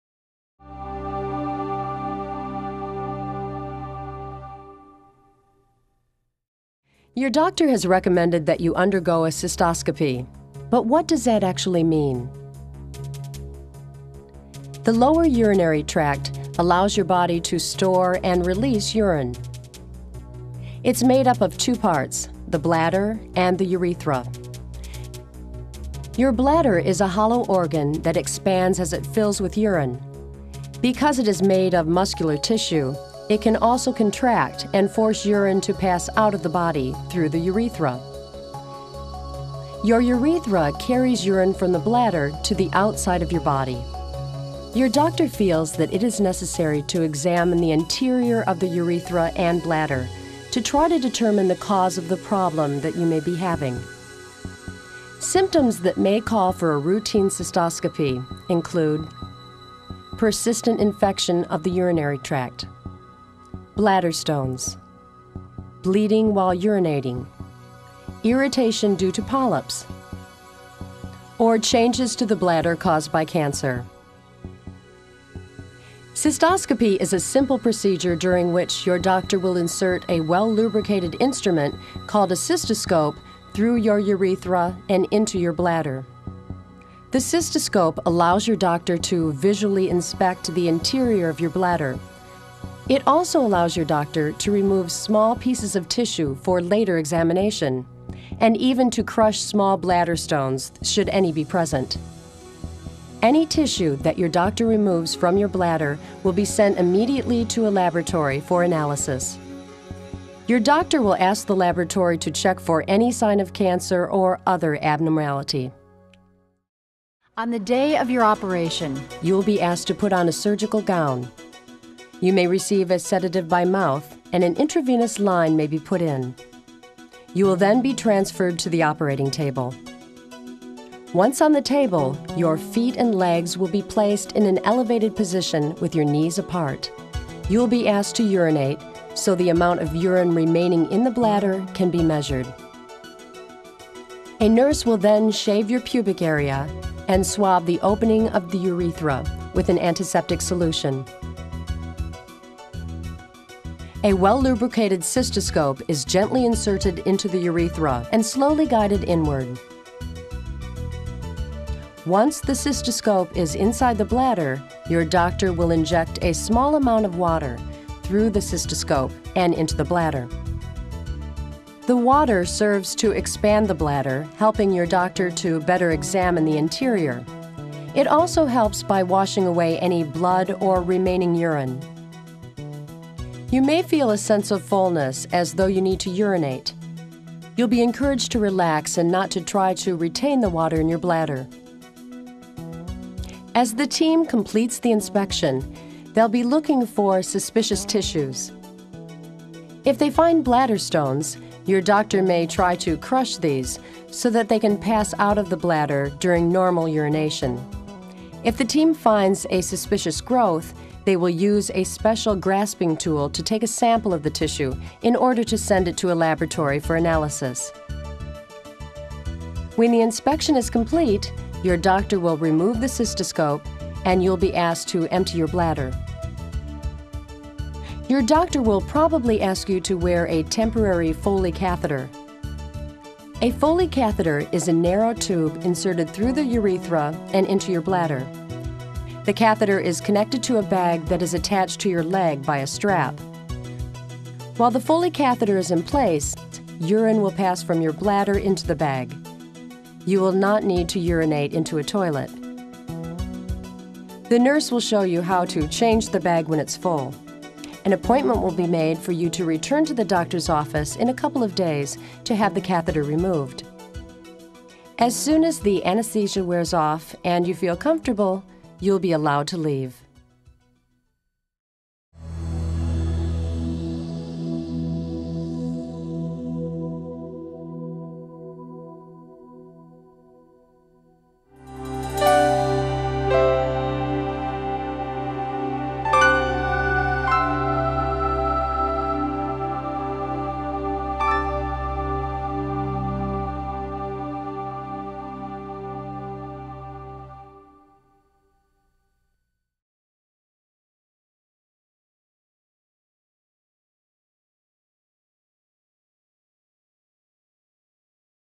PreOp® Educación del Paciente Cystoscopy Female Vaginal
PreOp® Educación del Paciente
Educación del Paciente de la empresa
u médico le ha recomendado que se someta a una cistoscopia. Pero, ¿qué significa esto exactamente?
El tracto urinario inferior, permite a su cuerpo para almacenar y liberar la orina.
Se compone de dos partes, la vejiga y la uretra.
Su vejiga es un órgano hueco que se expande a medida que se llena de orina. Debido a que está hecha de tejido muscular, que también pueden contraer y la fuerza para pasar la orina fuera del cuerpo, a través de la uretra. La uretra transporta la orina desde la vejiga hasta el exterior de su cuerpo.
Su médico considera que es necesario examinar el interior de la uretra y la vejiga, para tratar de determinar la causa de un problema que usted pueda estar teniendo.

Los síntomas que pueden exigir una cistoscopia de rutina incluyen:

    * La infección persistente de las vías urinarias
    * Cálculos en la vejiga
    * El sangrado al orinar
    * La irritación debido a los pólipos, o
    * Cambios en la vejiga causada por el cáncer.

La cistoscopia es un procedimiento sencillo durante el cual su médico le insertará un bien lubricado, instrumento llamado cistoscopio a través de la uretra y en la vejiga.
El cistoscopio permite a su médico para inspeccionar visualmente el interior de su vejiga. También permite a su médico para extraer pequeños trozos de tejido para su examen e incluso después de aplastar a las pequeñas piedras en la vejiga, a nadie se le presente.
Cualquier tejido que su médico le remueve de su vejiga se enviará inmediatamente al laboratorio para su análisis. Su médico le preguntará el laboratorio para detectar cualquier signo de cáncer u otras anomalías.